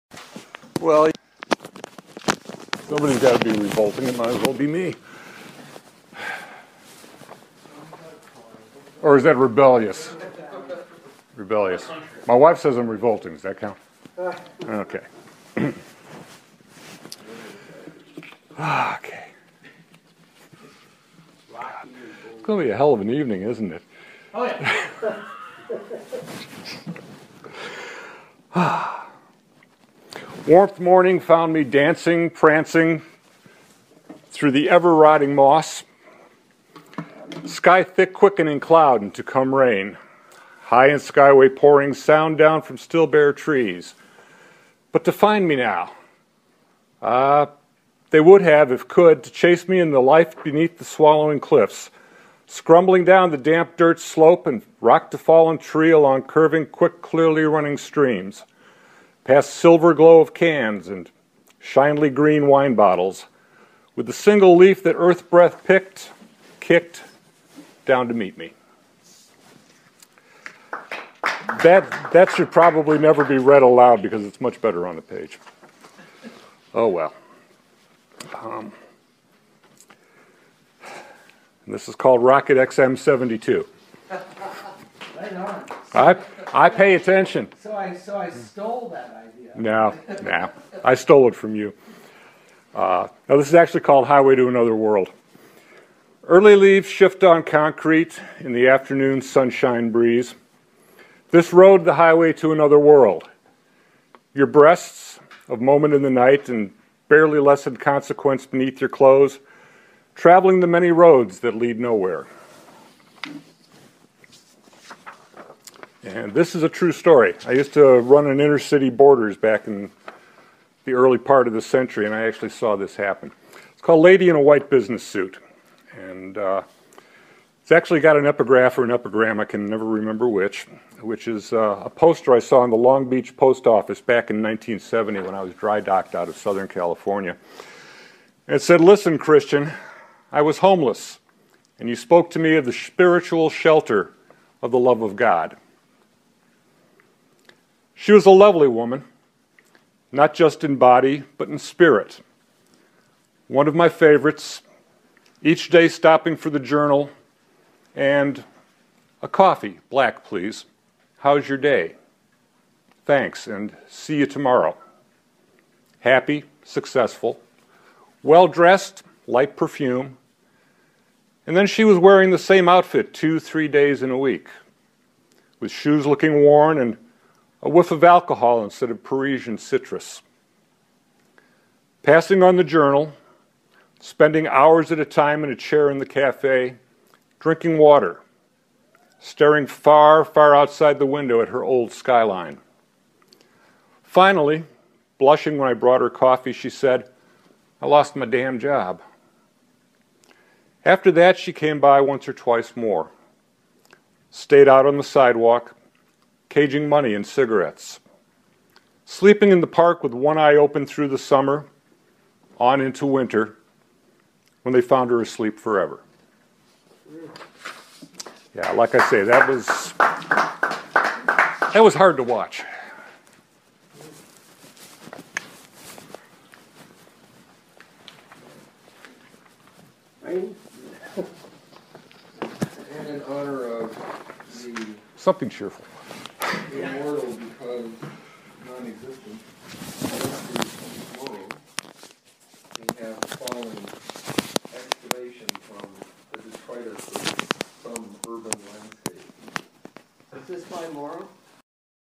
Lennart Lundh reads at Waiting for the Bus, recorded 20141201
Poems are (not in order):
Earth Breath
Highway to an Other World
Lady in White Business Suit

Photo by David Buddha-Hargarten

Closed captions aren't edited yet